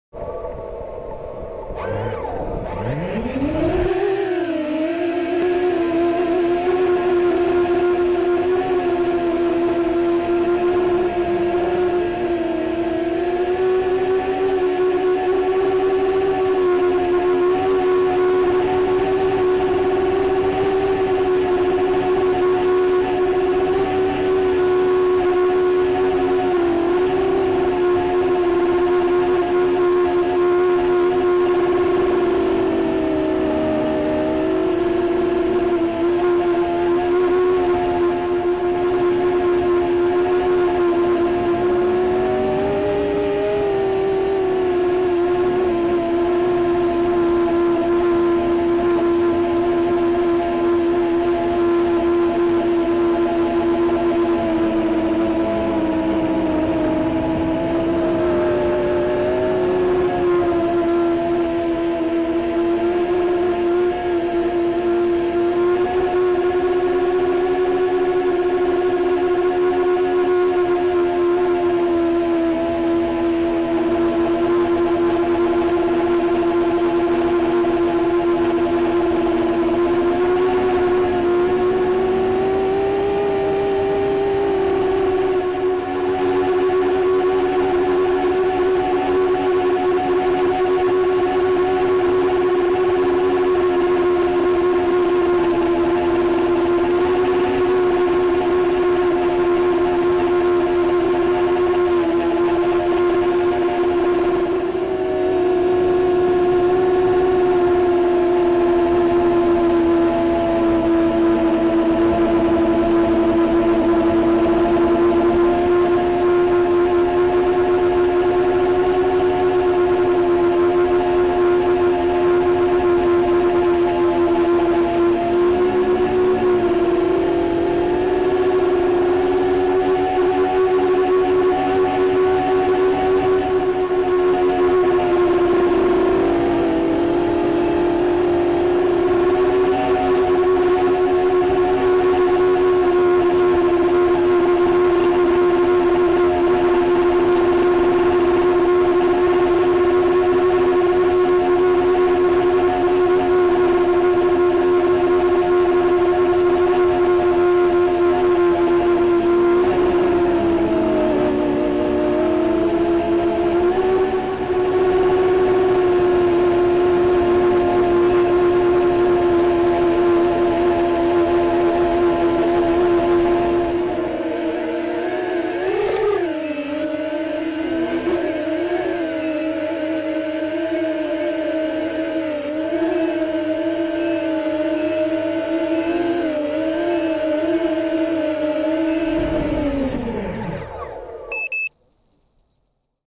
Longrange Miniquad Erstflug
Erster Flug mit Runcam 3 (WDR an) als HD und gleichzeitig FPV Cam auf dem 7" Longrange Quad.

FC: Omnibus F3 mit iNav und GPS
ESC: Racerstar 4 in 1 20a
Motoren: T-Motor Air200 2205/2000kv
Akku: 3s/7.000mAh LiIon (300g)
Rx: Crossfire Micro Rx
Vtx: Tbs Unify Pro 5V

Abfluggewicht: ca. 700g